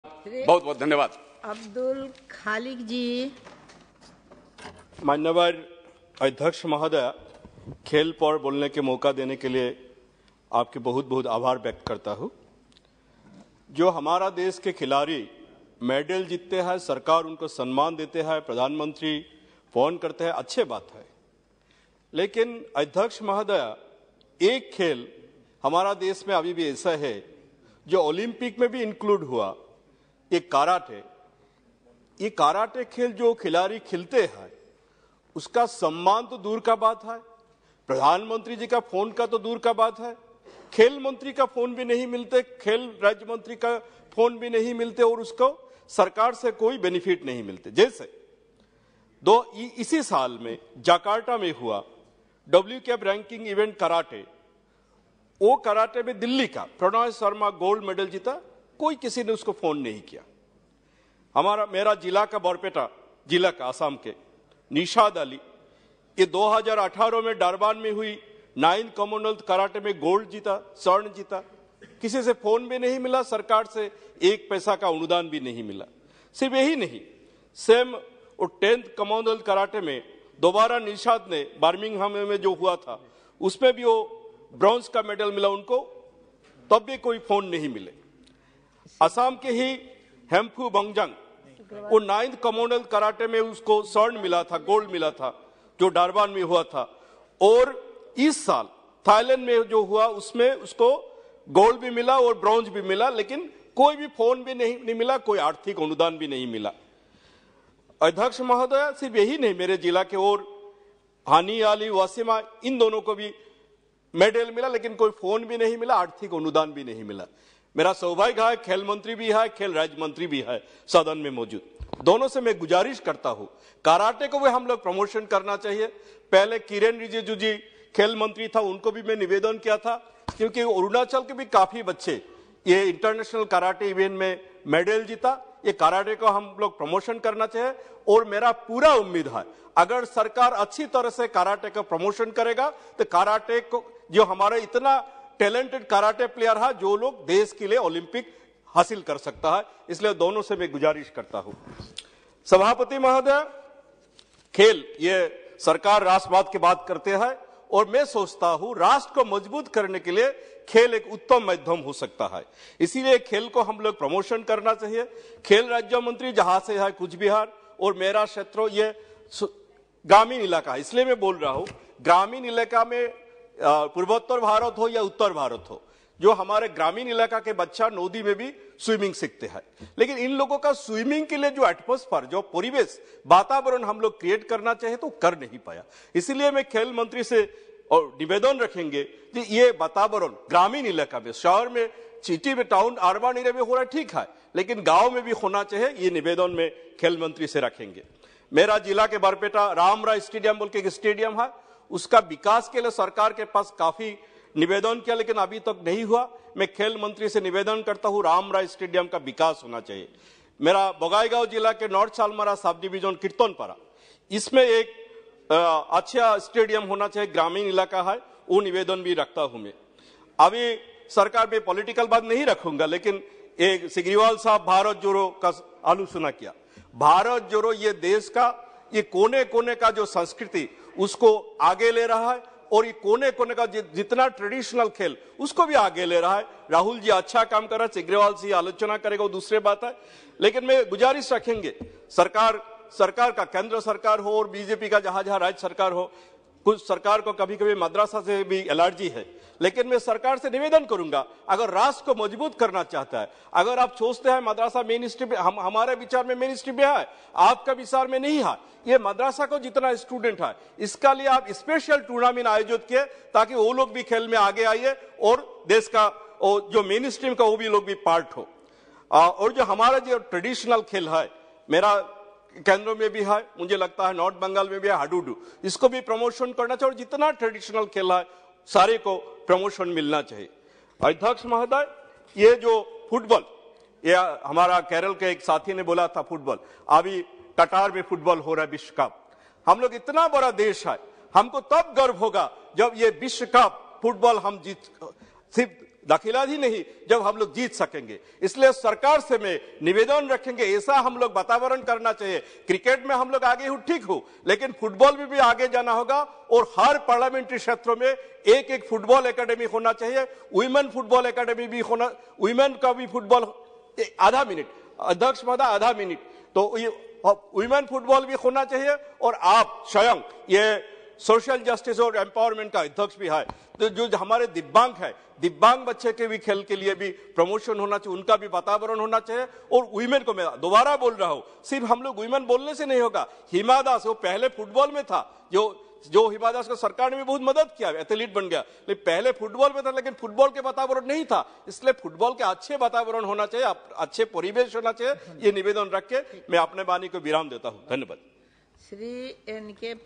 Abdul Khaleque| Discussion under Rule 193 on need to promote sports in India
Discussion under Rule 193  on the need to promote sports in India and steps taken by the Government in this regard raised by Gaurav Gogoi on the 31st March, 2022.